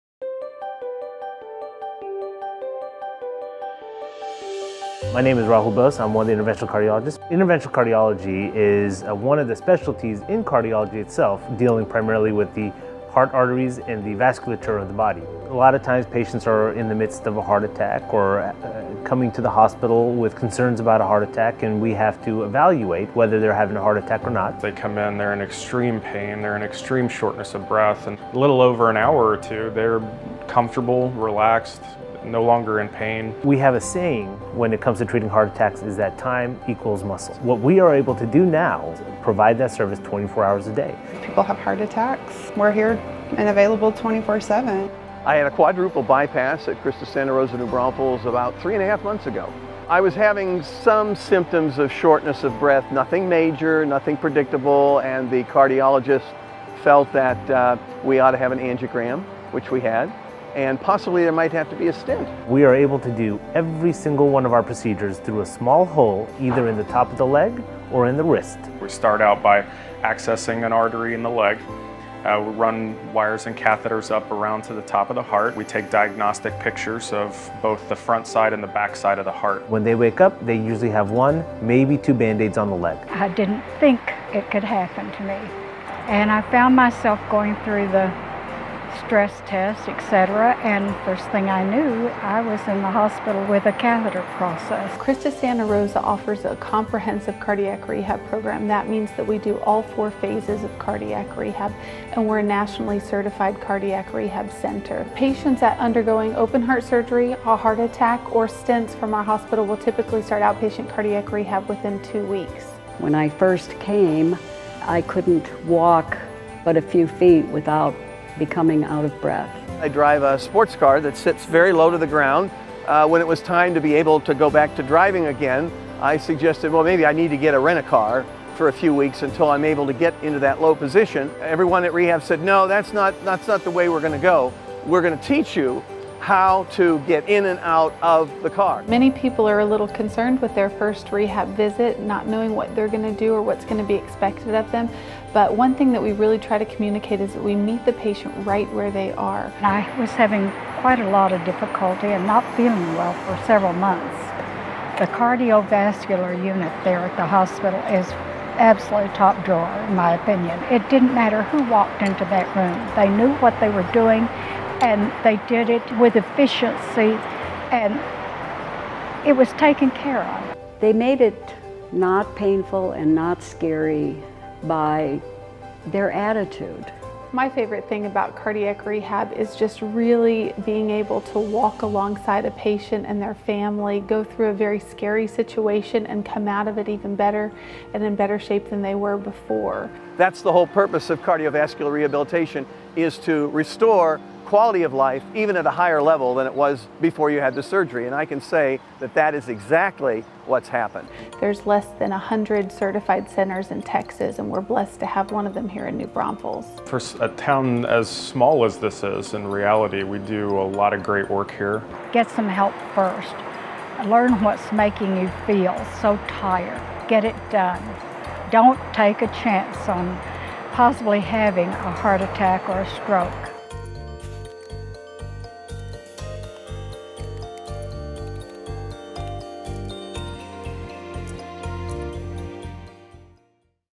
Cardiovascular Care in the Heart of New Braunfels
The Cardiovascular Center at CHRISTUS Santa Rosa Hospital - New Braunfels provides quality service, top-tier technology, and compassionate care for a patient-friendly experience delivered around the clock.

Our dedicated Center is chest-pain accredited by the Society of Cardiovascular Patient Care and is fully equipped to provide the highest quality of cardiovascular care to our patients.